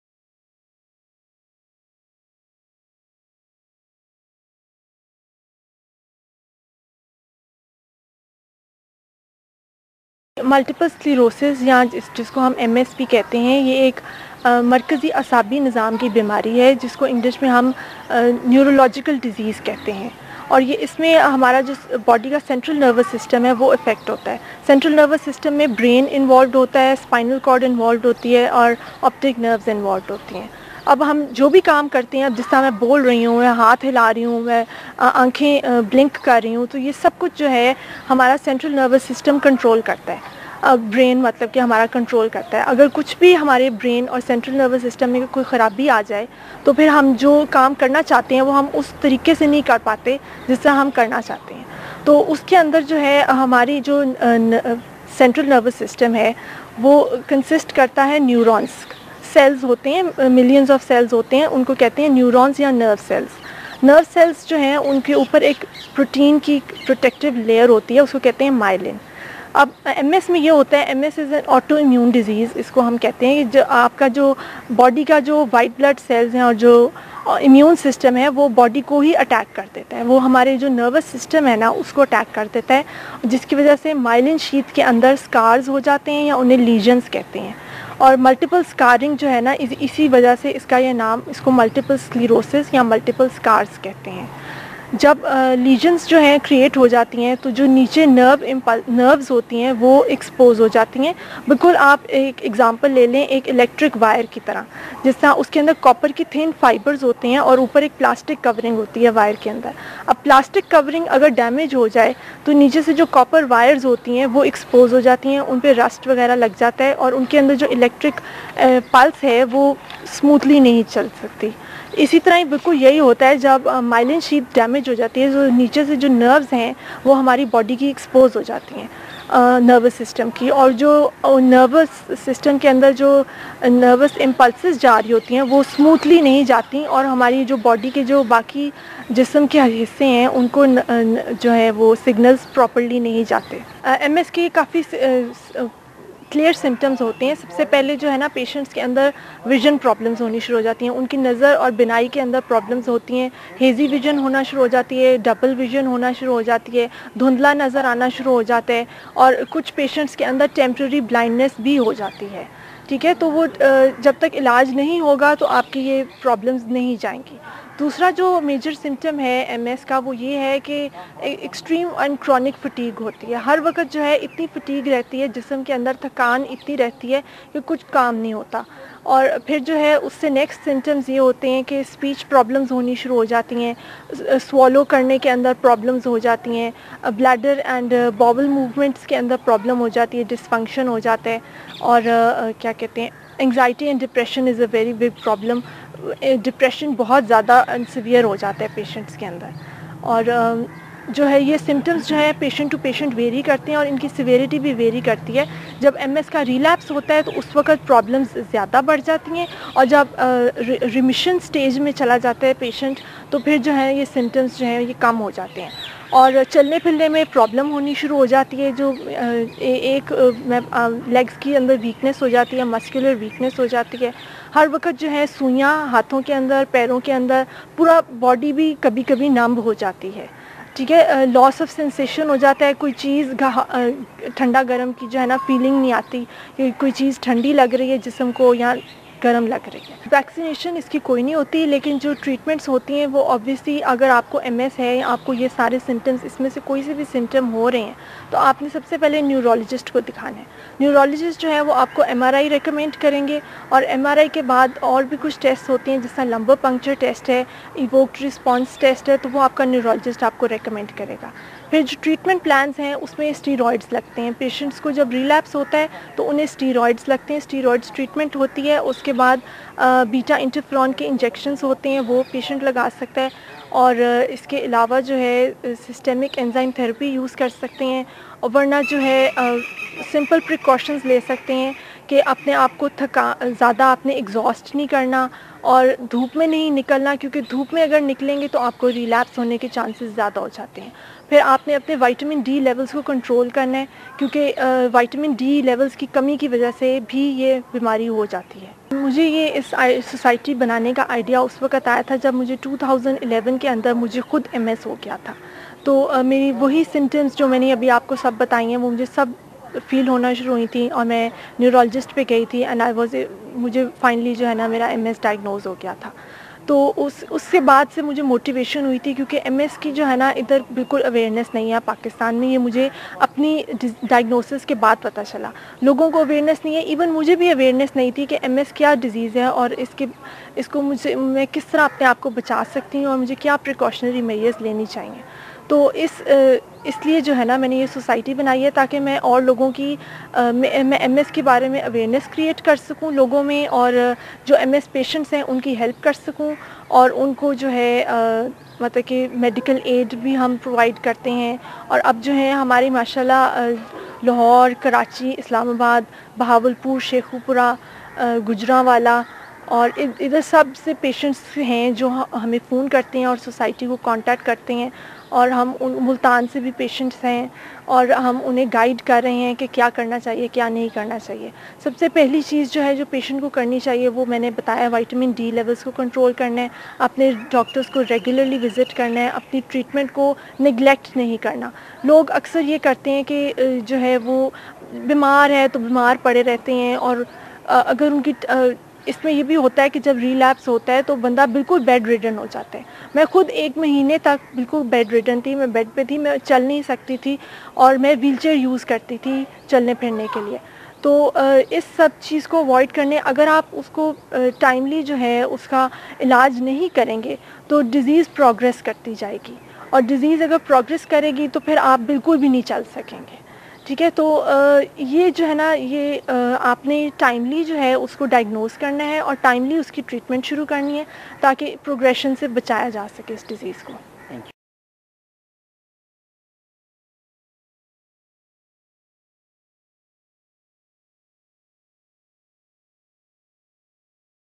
What is MS? Find out about MS - Afroz Syed Interview
The Society for Multiple Sclerosis Patients in Pakistan raised awareness on World MS Day 2014. Watch the video to find out what MS is, what are the symptoms of MS and what treatments are available.
Speaker: Afroz Syed (President, Society for Multiple Sclerosis Patients in Pakistan)